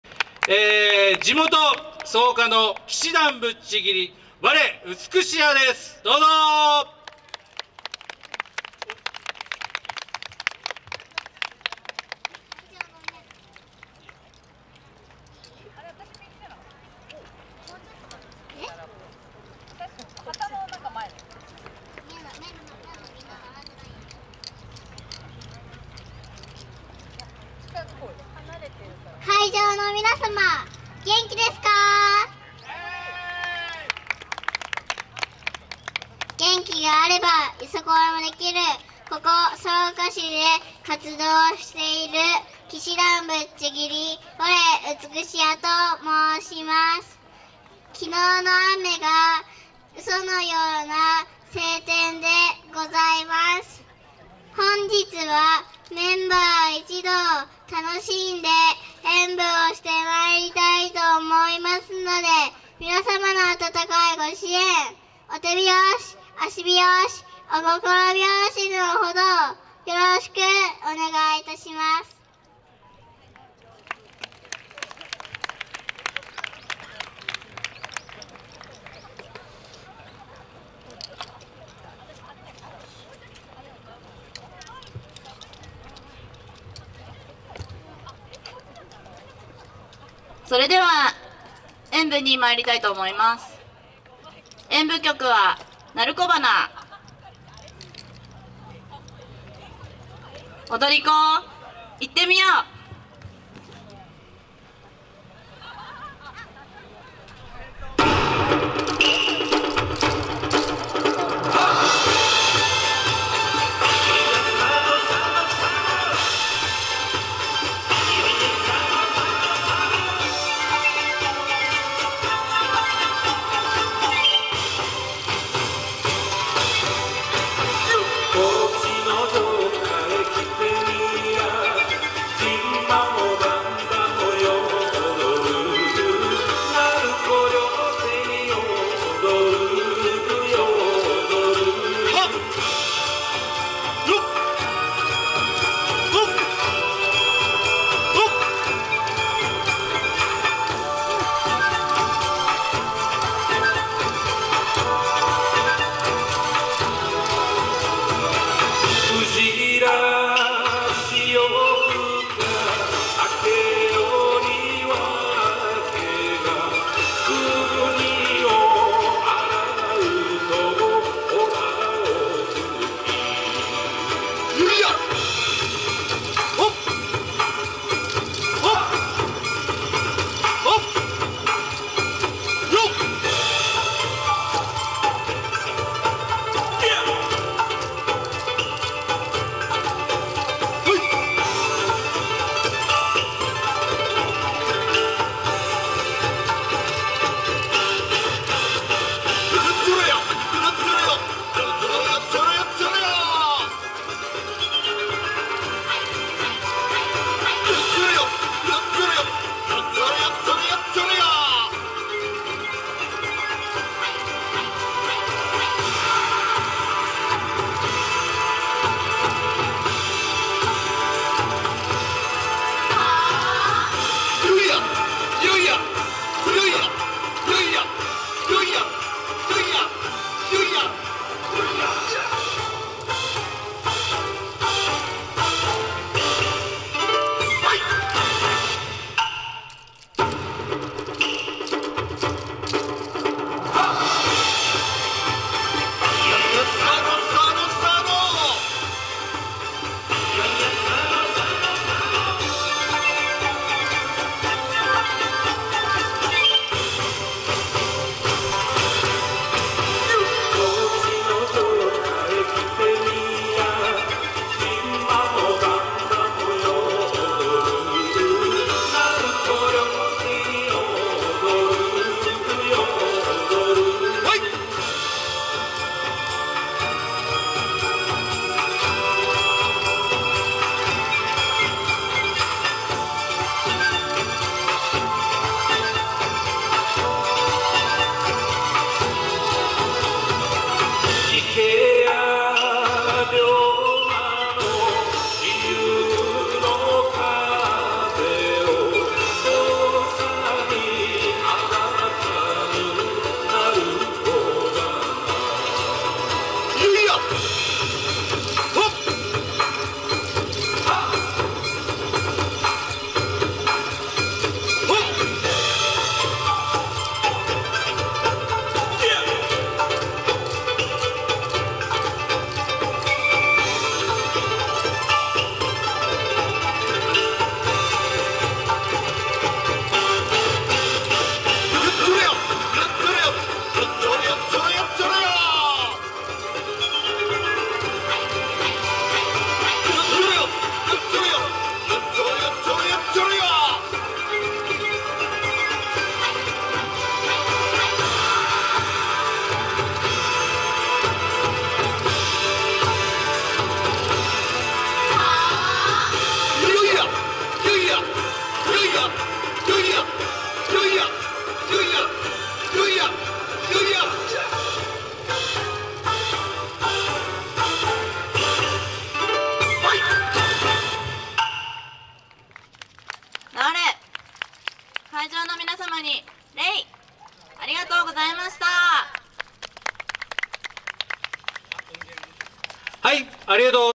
オープニング 旗士團☆舞散旗人 吾美屋【4K】／ウォーターフロント会場／YOSAKOIそうか2024（第32回草加商工会議所まつり）〜相思草愛〜
2024/11/03（令和6年11月3日）
第32回草加商工会議所まつり
YOSAKOIそうか2024〜相思草愛〜
【会場】まつばら綾瀬川公園
【主催】草加市よさこい振興会
【共催】草加商工会議所